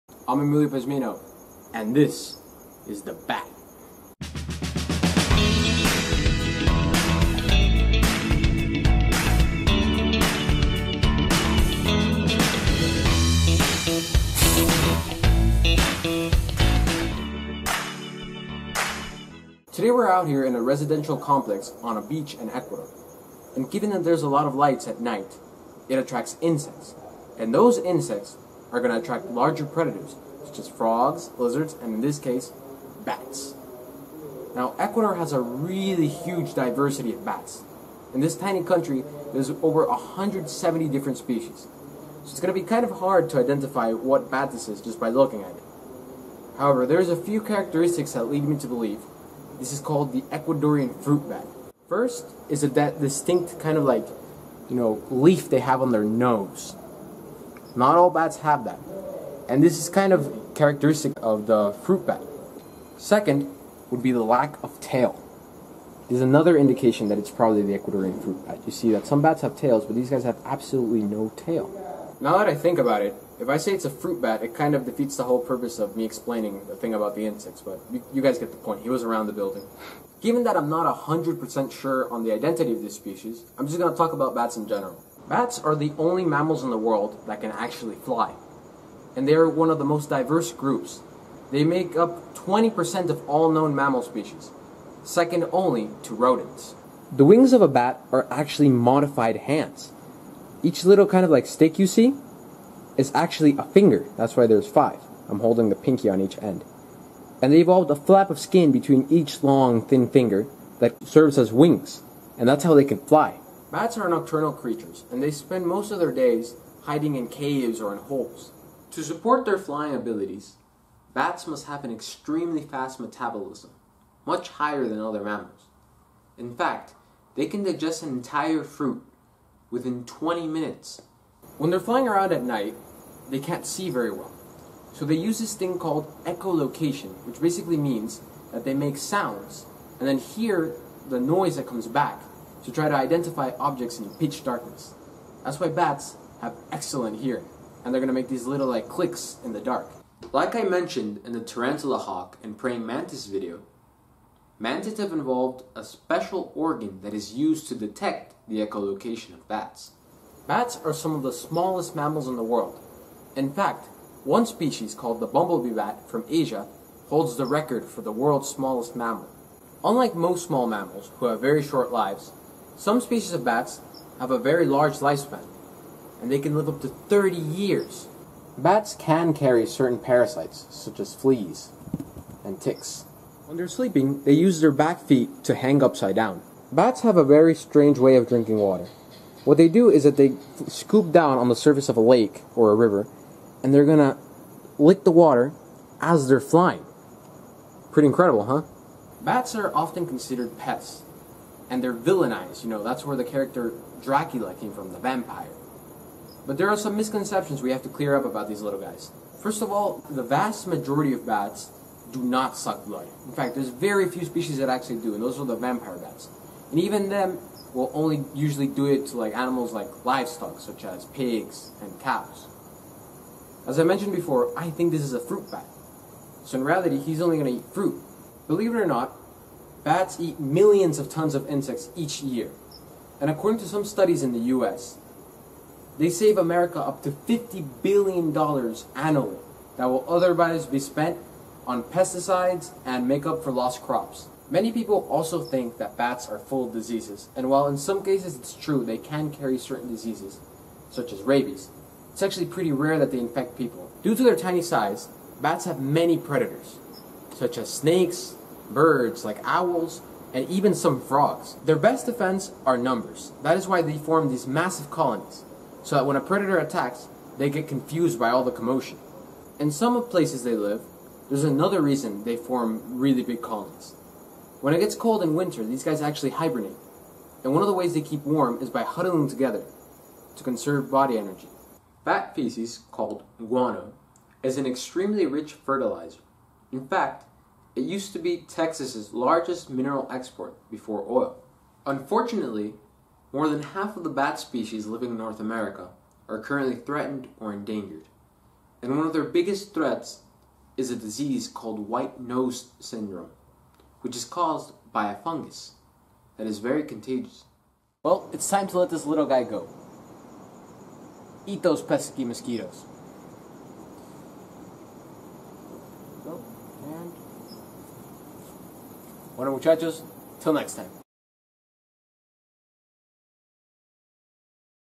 The Ecuadorian fruit bat
Today on Animal Encounters, we manage to catch the world's only flying mammals, and prove why bats are interesting and even helpful creatures, not "flying rodents" as considered by many.